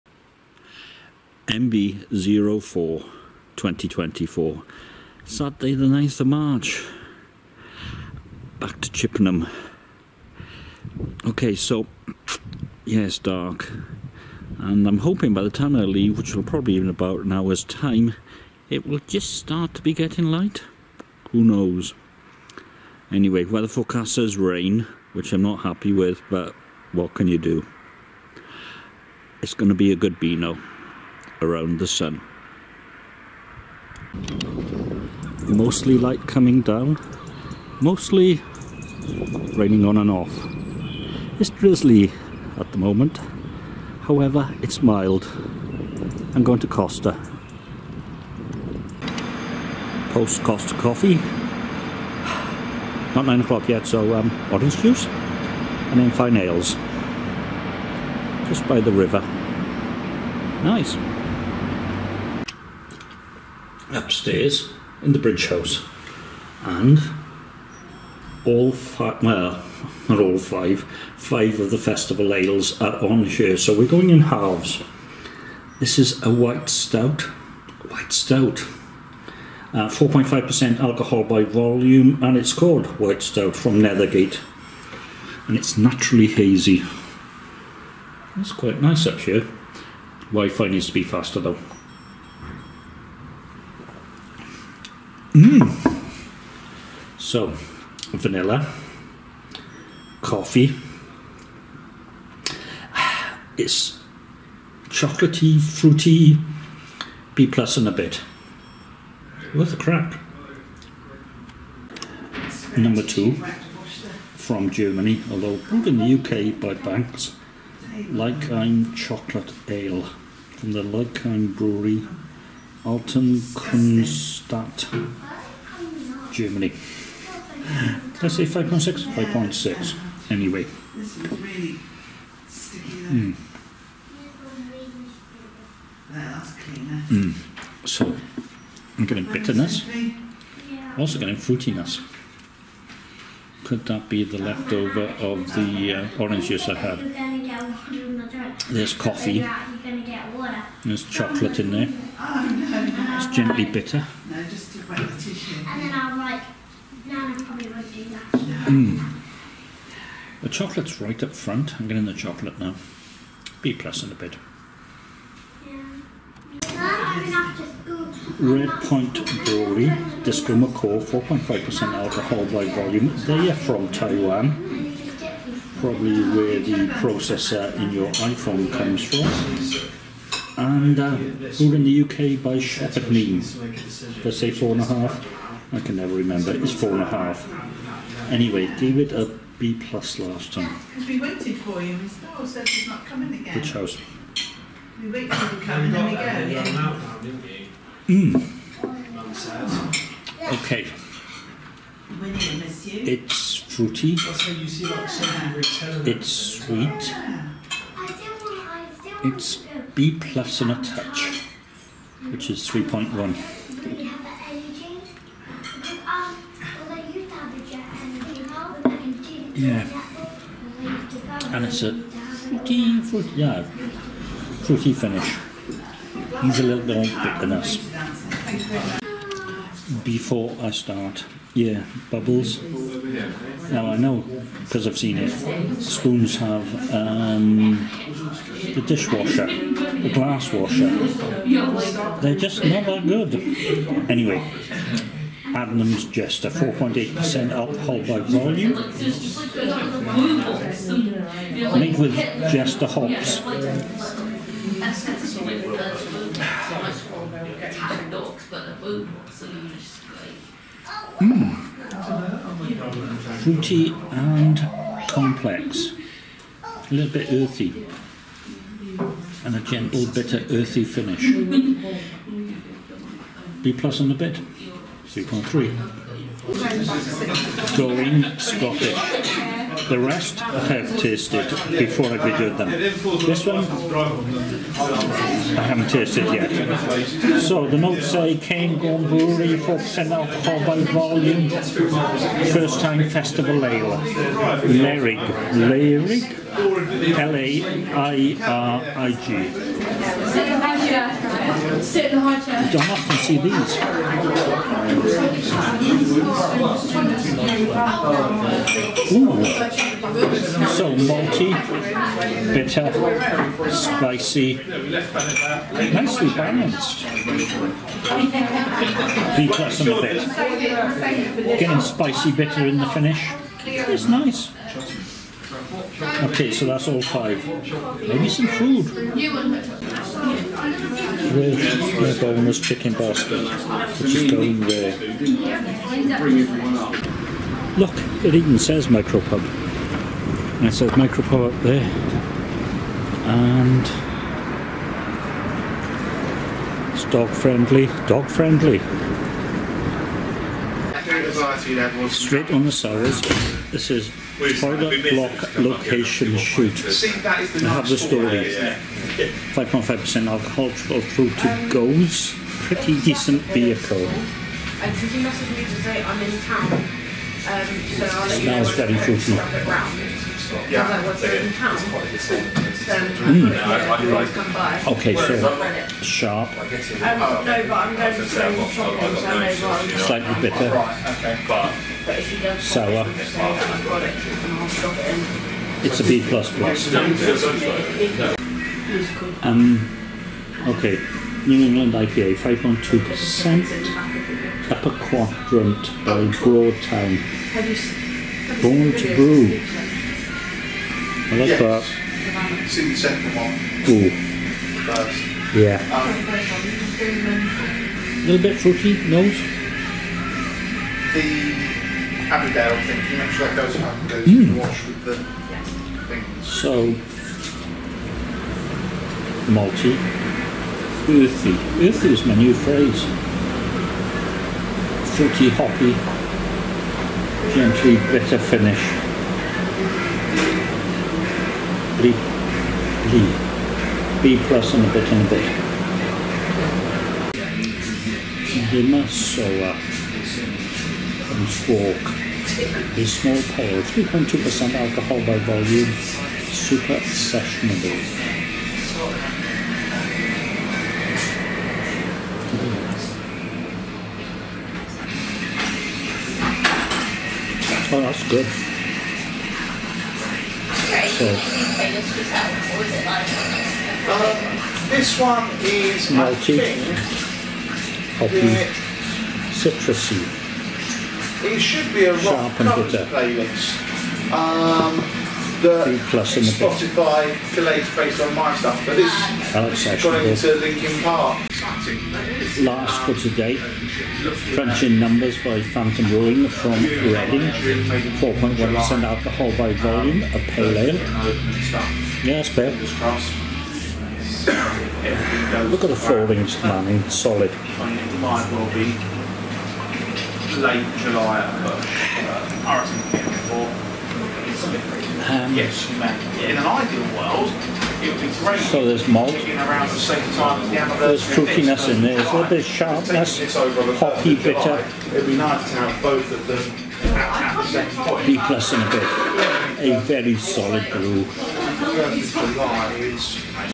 Five and Dry
Of course the journey to Chippenham was damp. However, it was mostly light, which is a major positive. Costa then Spoons, The Bridge House. Five Fest Ales all reviewed and all halves.  Sitting upstairs, overlooking the river. A very pleasant morning.

A boneless Chicken basket for lunch and then over to the Prince Of Wales. Dogs, Fine Ales and news of a new Craft Pub in July.  Chippenham is becoming my new Portishead.  Quality Beanoing.